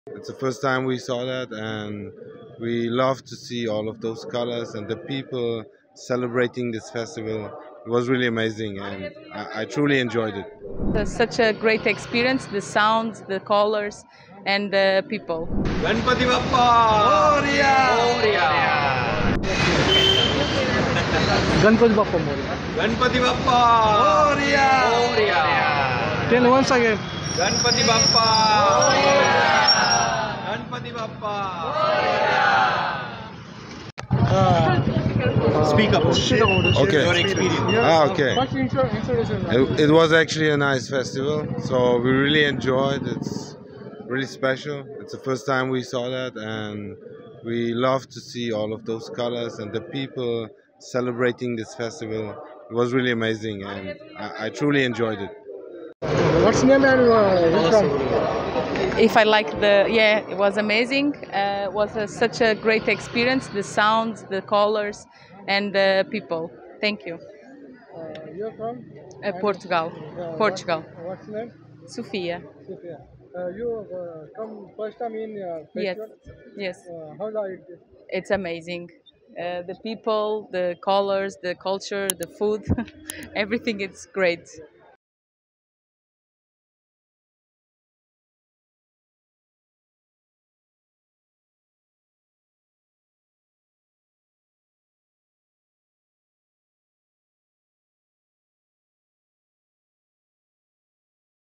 Pune Ganesh Visarjan | विसर्जन मिरवणूक : परदेशी नागरिकांच्या भावना काय? | LetsUpp Marathi
पुण्यात मानाच्या गणपतीच्या मिरवणुका उत्साहात सुरु झाल्यात. गुलाल आणि फुलांची उधळण करत जल्लोष सुरु आहे. दरम्यान मिरवणुकीत परदेशी नागरिकांची हजेरी पाहायला मिळाला.